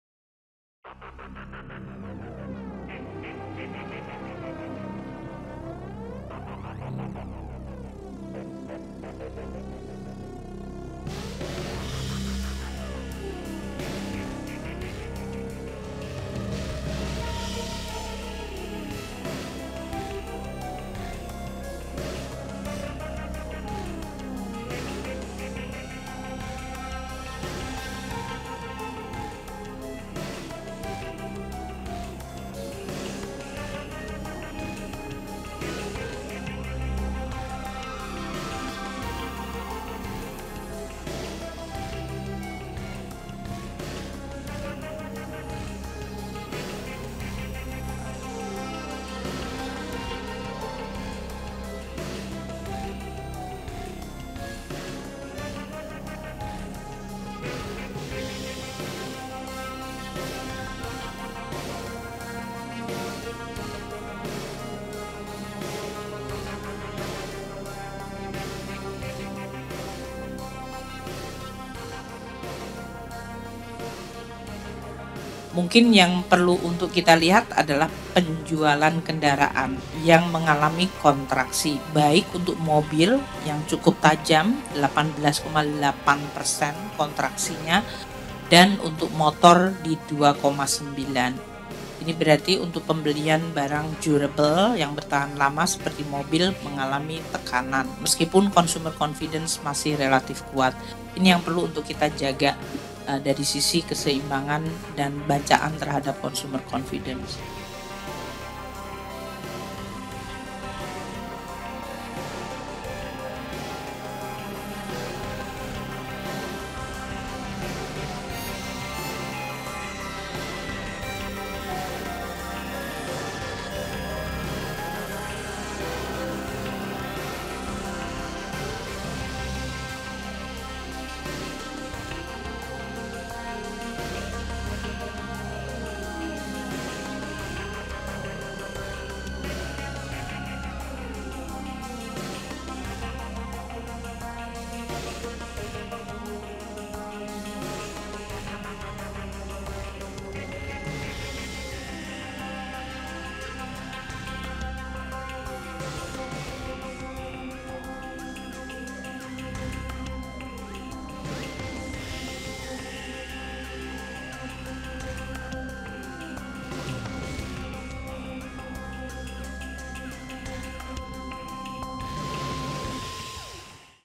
Menkeu Ungkap Penjualan Mobil dan Motor Merosot Awal 2024
Menteri Keuangan RI Sri Mulyani Indrawati menyatakan bahwa penjualan mobil dan motor mengalami kontraksi atau penurunan pada Februari 2024. Walaupun penjualan kedua jenis kendaraan tersebut mengalami kontraksi, namun mantan Direktur PelaksanaBank Dunia itu menyebut, indeks kepercayaan konsumen (IKK) per Februari 2024 masih relatif stabil di level 123,1. Adapun Indeks Kepercayaan Industri (IKI) masih ekspansif pada Februari 2024.